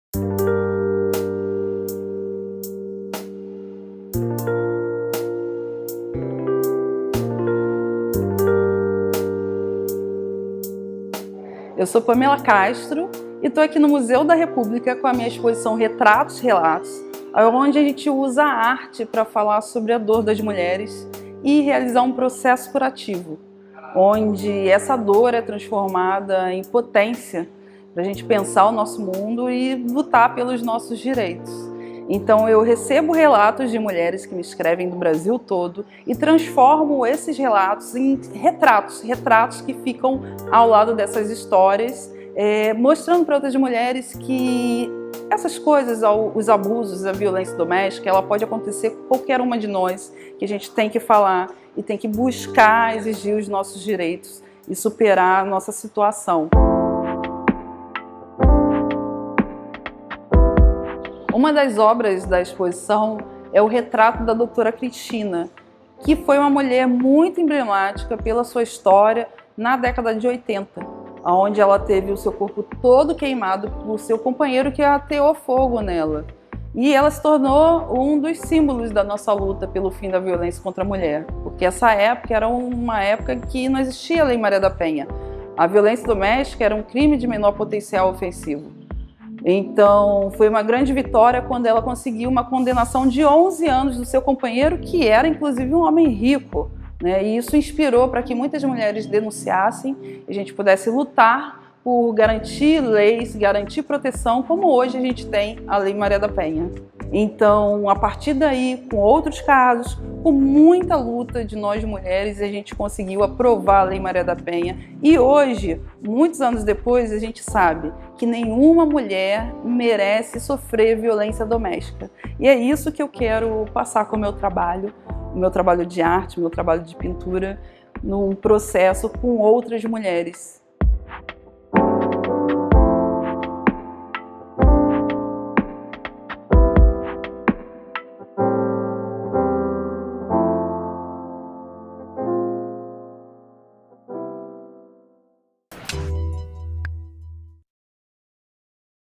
RETRATOS RELATOS DE PANMELA CASTRO
A artista visual Panmela Castro apresenta sua exposição "Retratos Relatos", que usa a arte para falar sobre a dor das mulheres que sofreram abusos e violências.